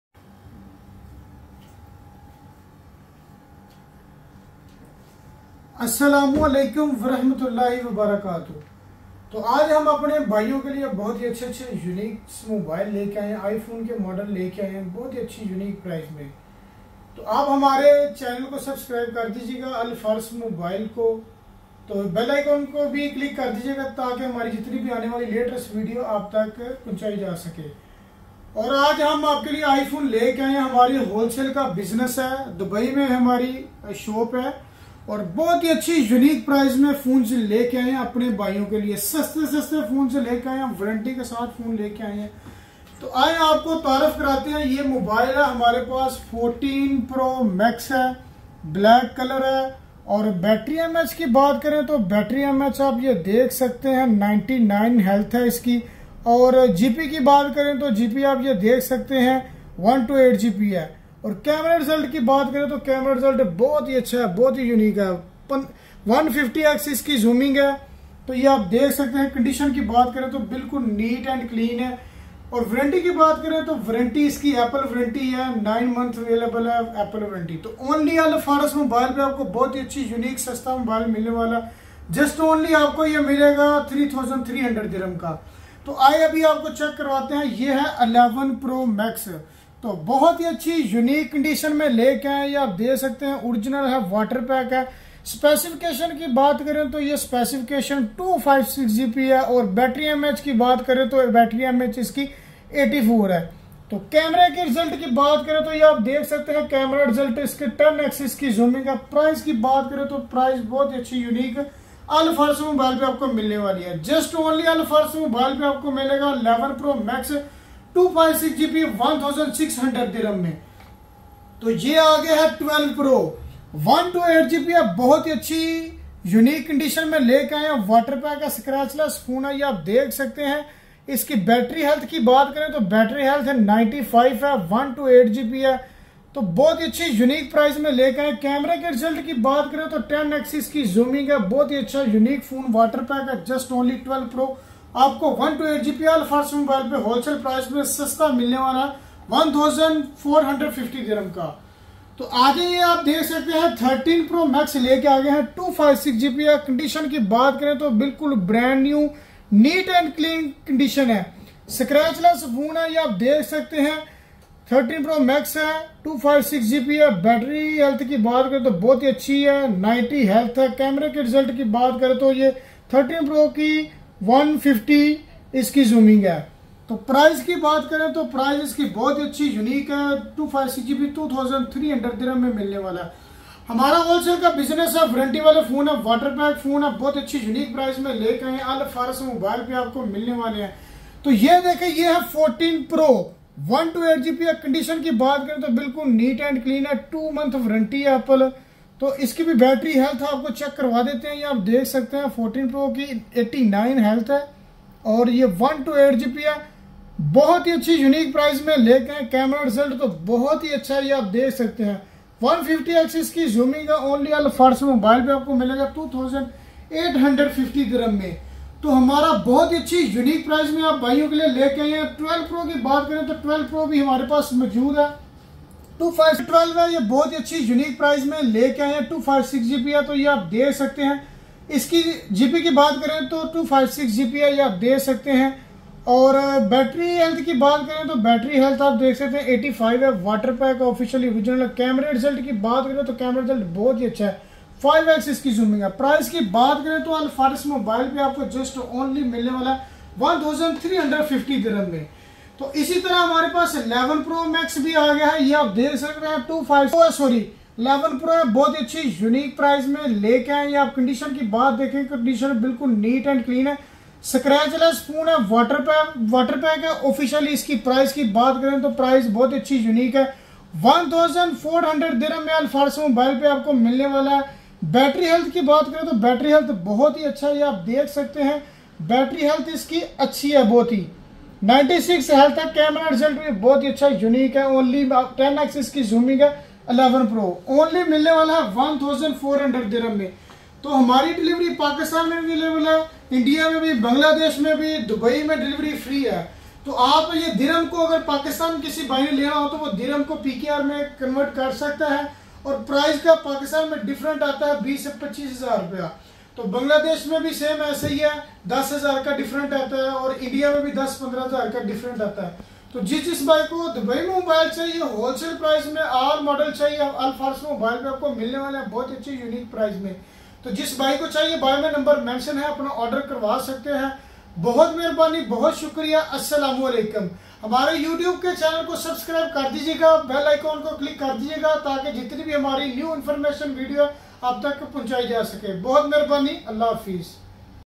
IPHONE 14 PRO MAX IN PRICE DUBAI | 13PRO | 12 PRO MAX | 11 PRO MAX | DUBAI VLOGS
IPHONE 14 PRO MAX IN PRICE DUBAI
11 PRO 256GB 1400 AED
11 PRO max 256GB 1600 AED
13PRO 128GB 2100AED
13 PRO 256GB 2200 AED
13 PRO max 128GB 2400 AED
13 PRO max 256GB 2500 AED
12 64GB 1250 AED
12 128GB 1300 AED
12 256GB 1400 AED
12 PRO 128GB 1400 AED
12 PRO 256GB 1600 AED
12 PRO max 128GB 2000 AED
12 PRO max 256GB 2100 AED
14 PRO 128GB 2800 AED
14 PRO 256GB 2900 AED
14 PRO max 128GB 3300 AED
14 PRO max 256GB 3400 AED
Al Fares Mobile deria Dubai